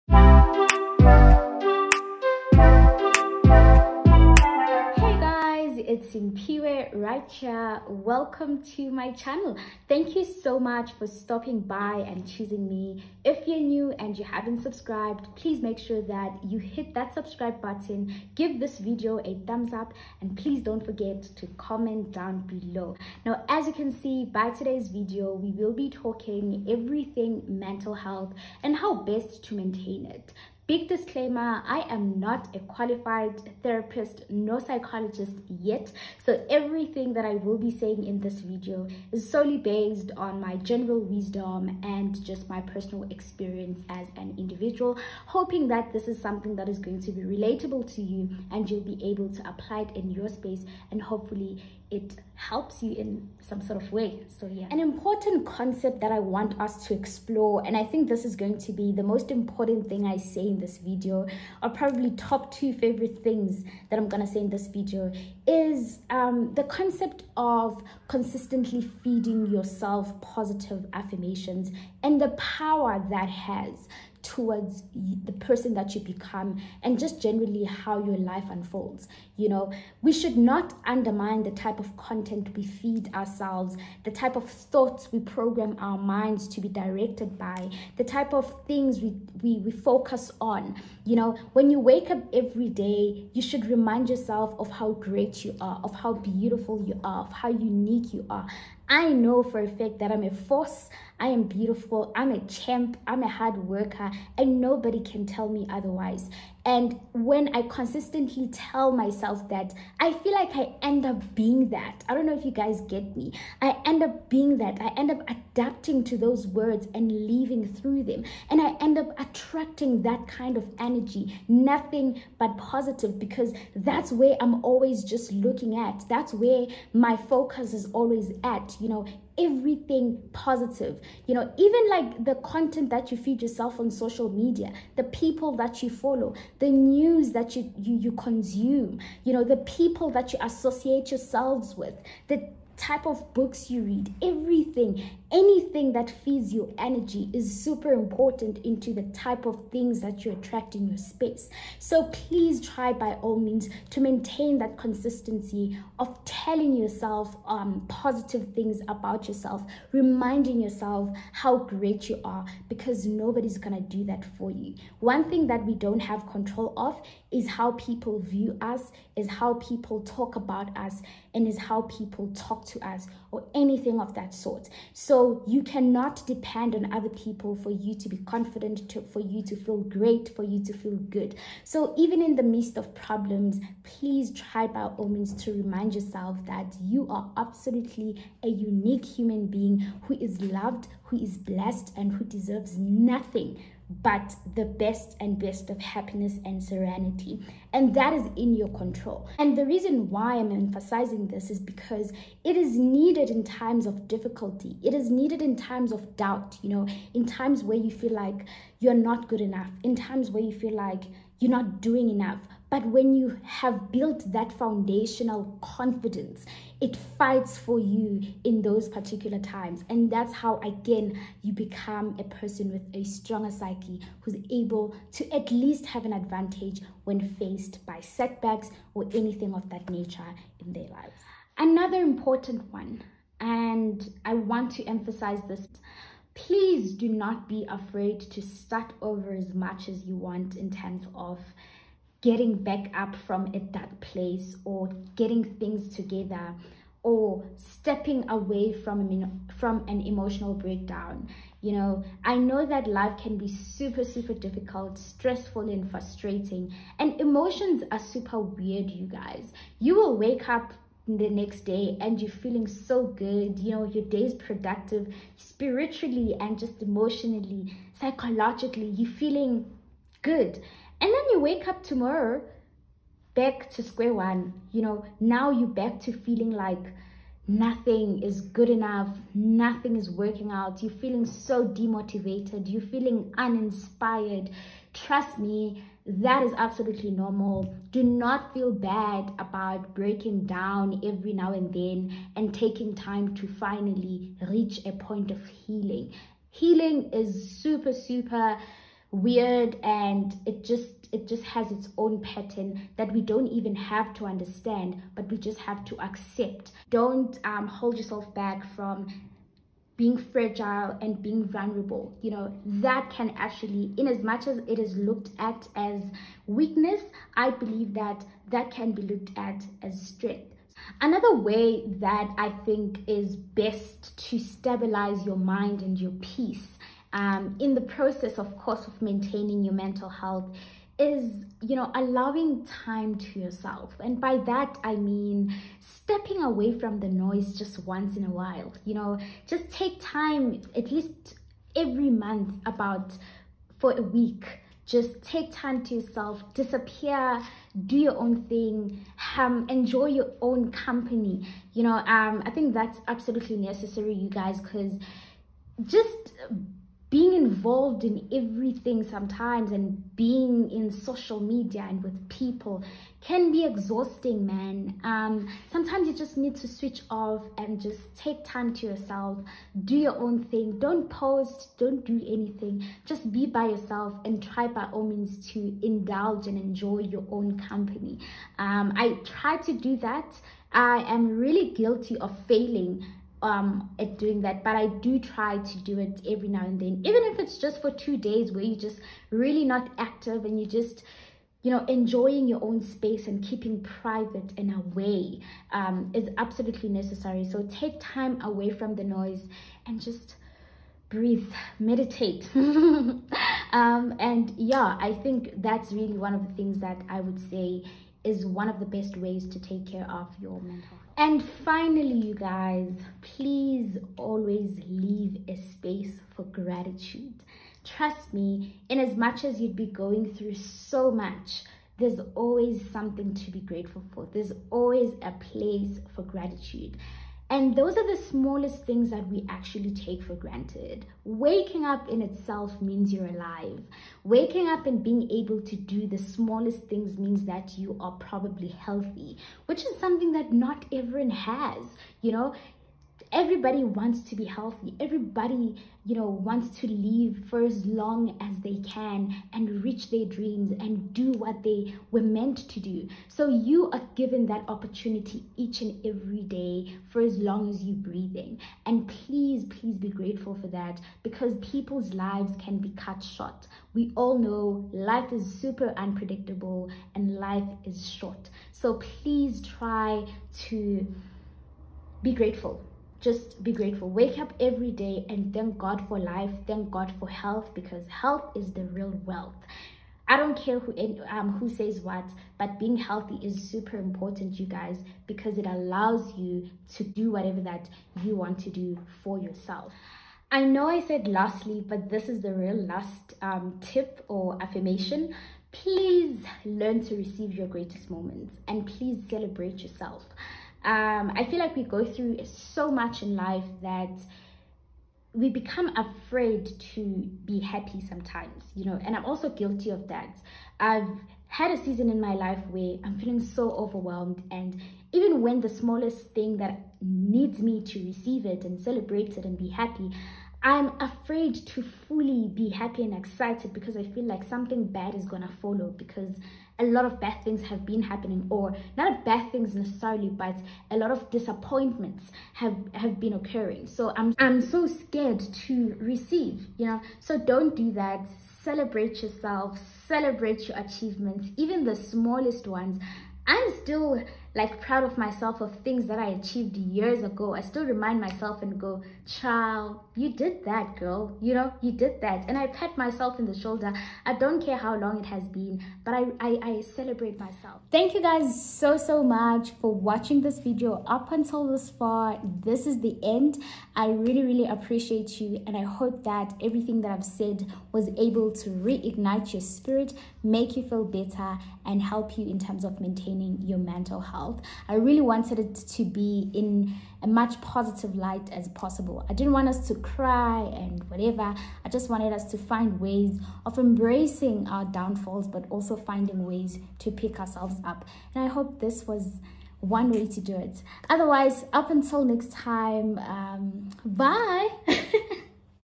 Mental Health Chit-Chat 💕|| Affirmations ||SOUTH AFRICAN YOUTUBER🇿🇦
Hey Phiwe Clan ! I’m back with another video ☺️and today we are talking everything mental health and how to maintain it . I hope you will enjoy this video ❤️